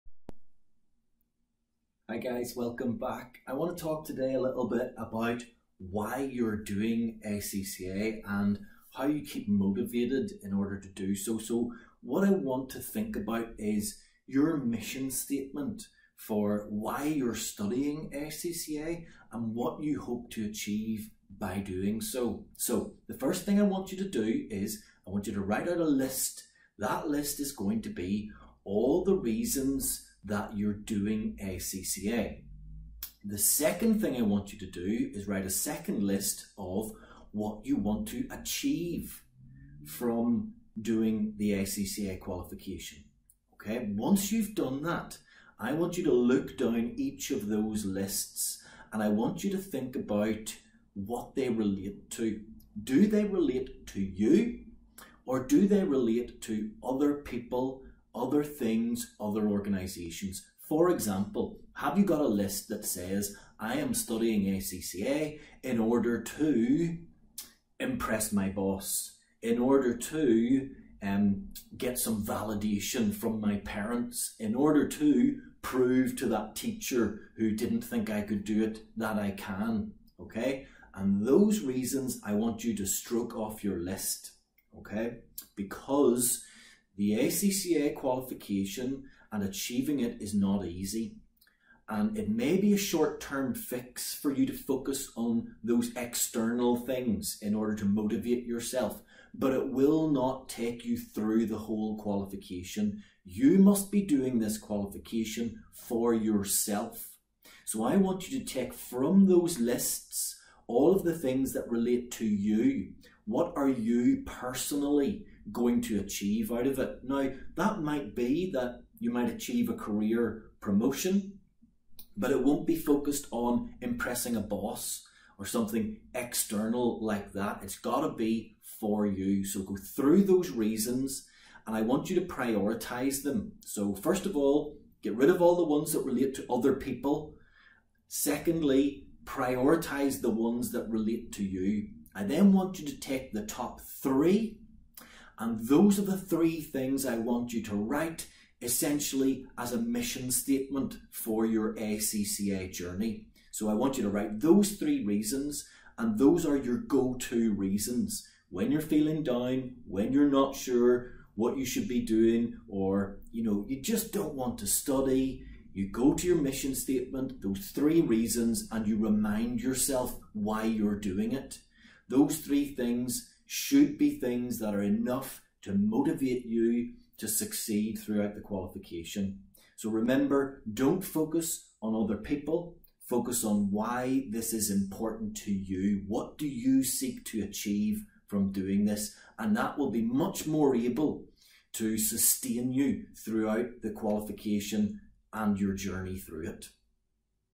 Create your ACCA Mission Statement
Keep yourself motivated to complete the ACCA qualification with these simple steps.